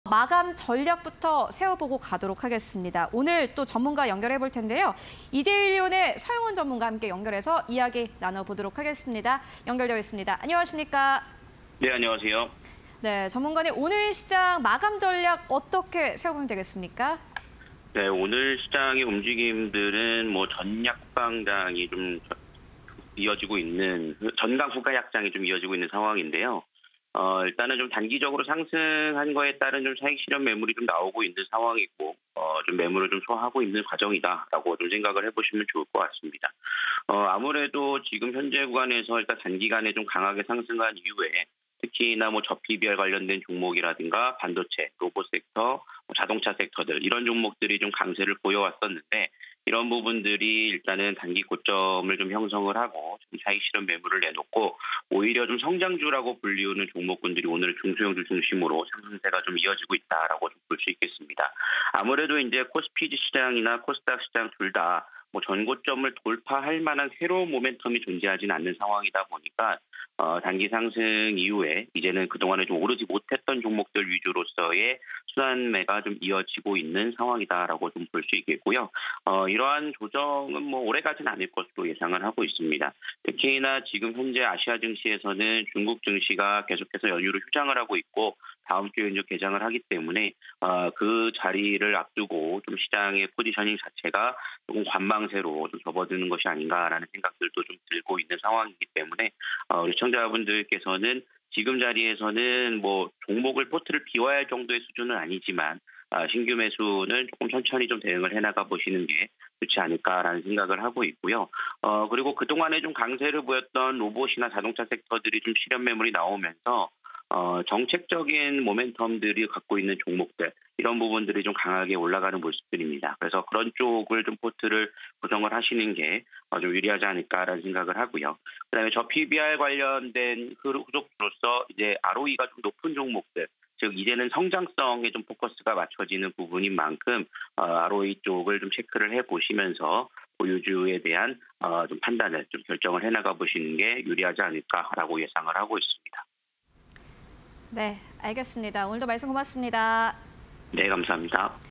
코스피, 단기 상승에 따른 매물 소화 과정_서용원 마감 전략 (20240215)
Q. 오늘 장 마감 흐름은?
- 코스피, 단기 상승에 따른 매물 소화 과정
- 로봇‧반도체‧자동차주 차익실현 매물 출회
- 단기 조정 후 시장 안정화 전망